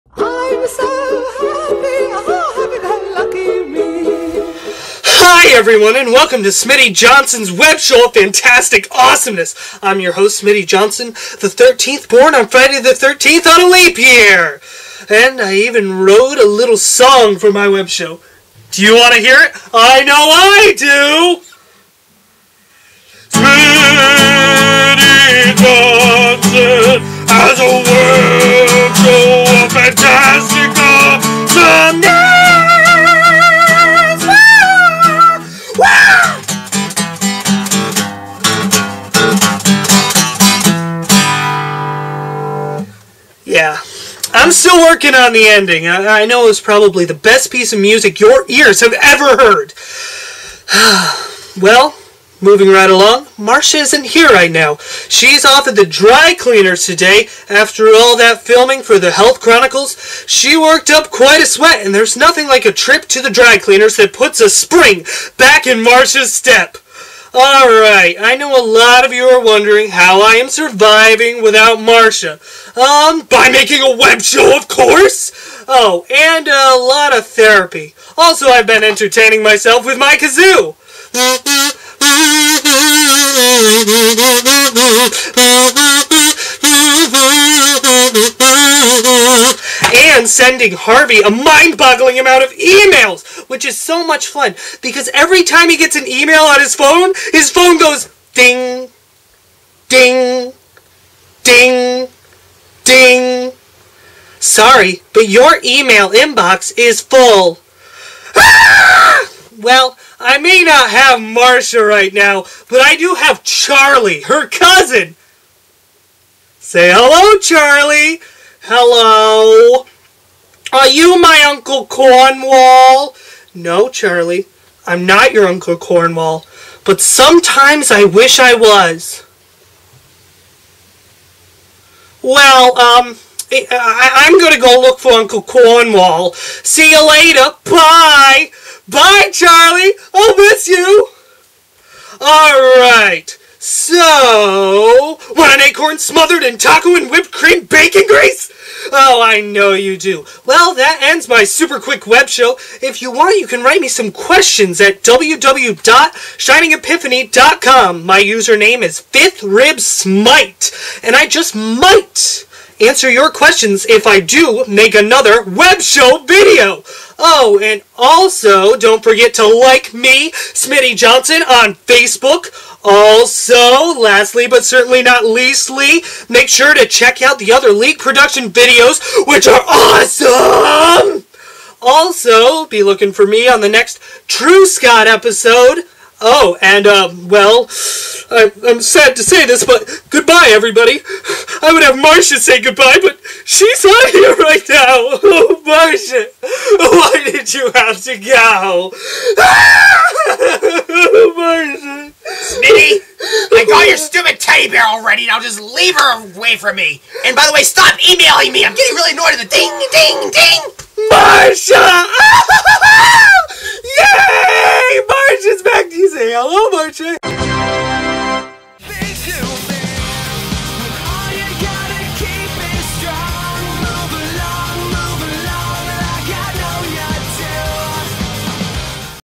Smitty Johnson's Web Show of Fantastic Awesomeness
Hey guys!  Smitty here with my web show of fantastic awesomeness!  I just would like to say, that Marsha is at the cleaners, which makes me sad and because of this, I have been working on this super awesome theme song! Ahahah!!!  Well, bye everybody!